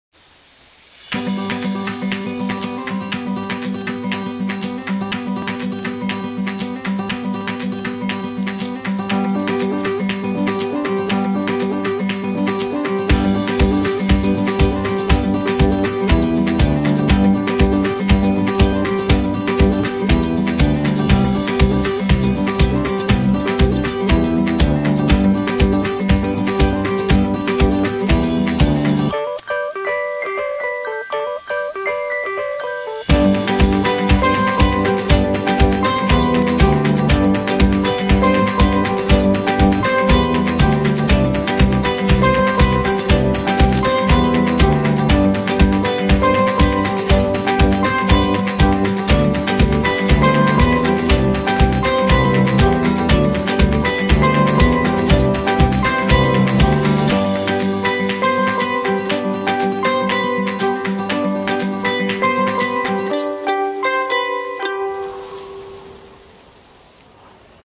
Painting grapes
The medium of this paiting is acrylic on canvas board.